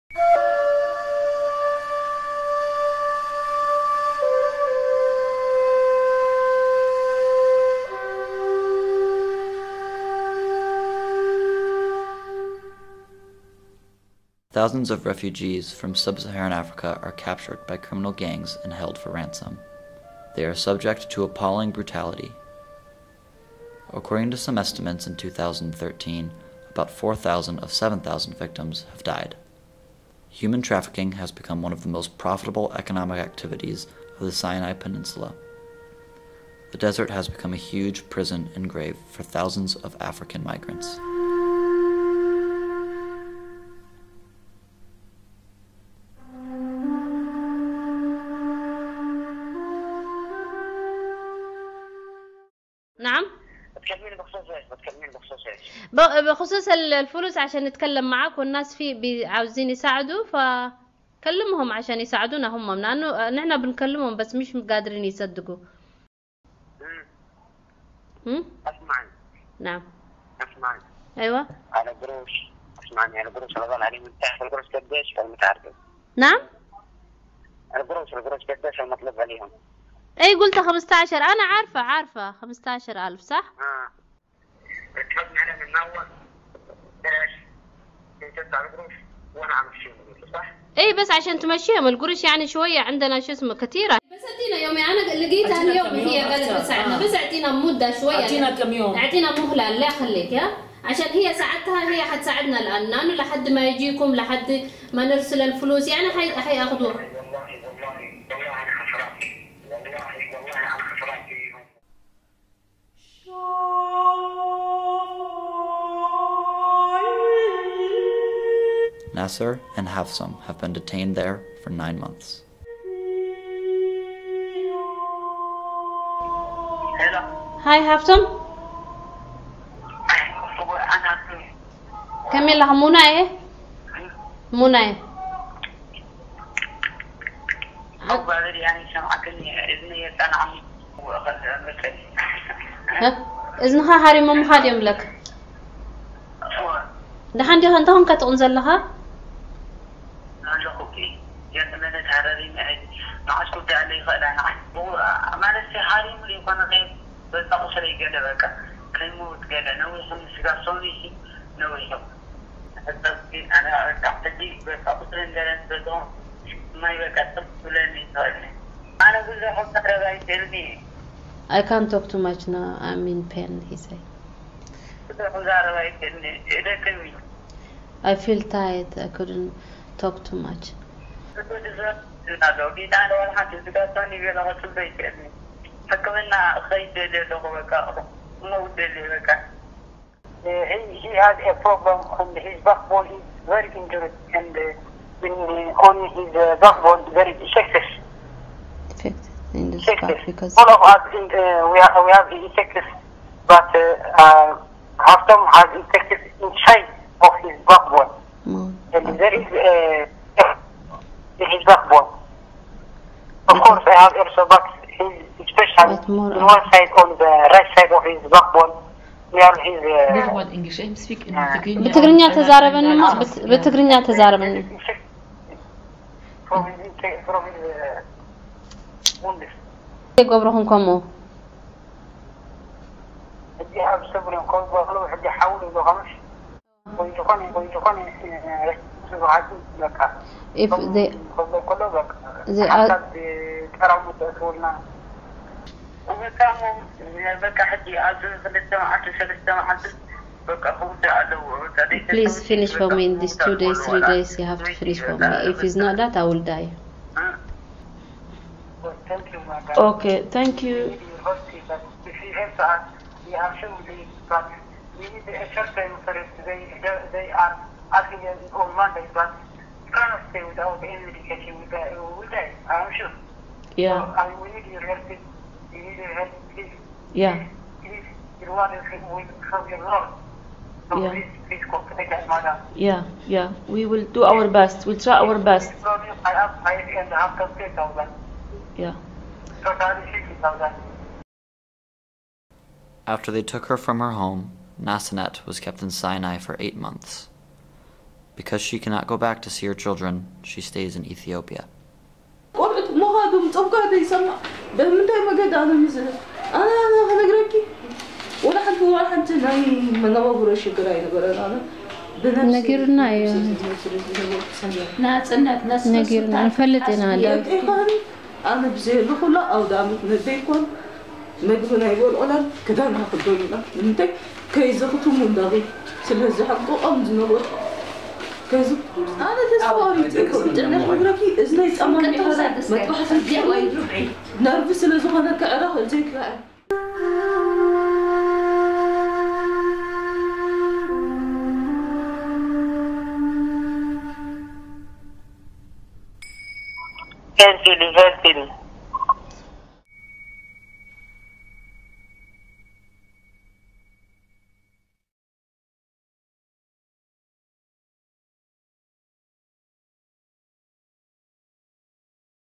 Sinai
Watch our short video to learn more about the plight of African migrants in the Sinai desert